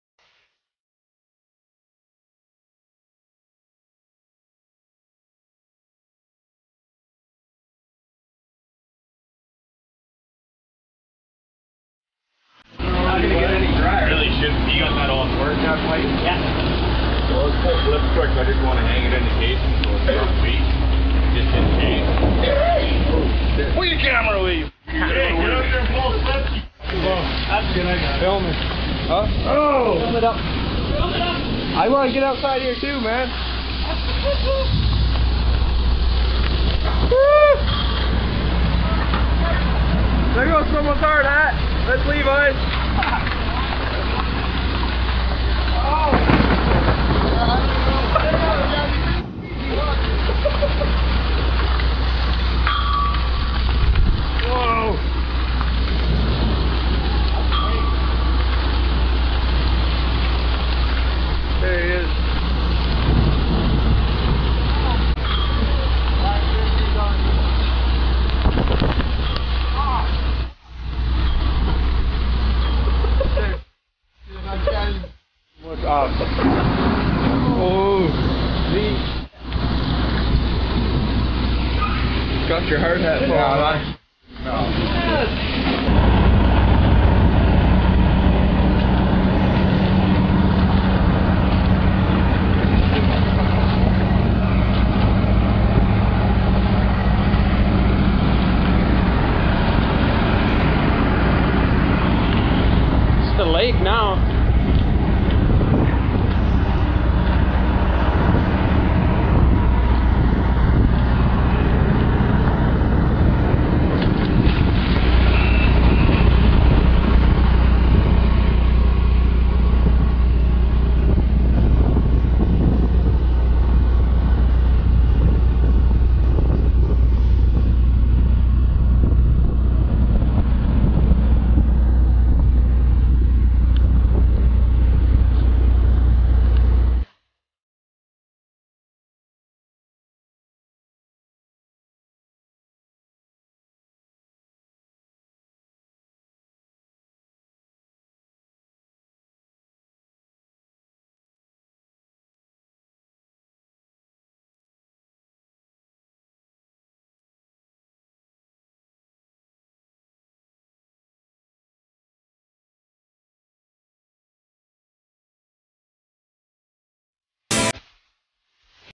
Rigging in a storm
we were running casing after drilling the hole and ine the middle of it we were hit with an awesome storm, you can see hail bouncing off everyone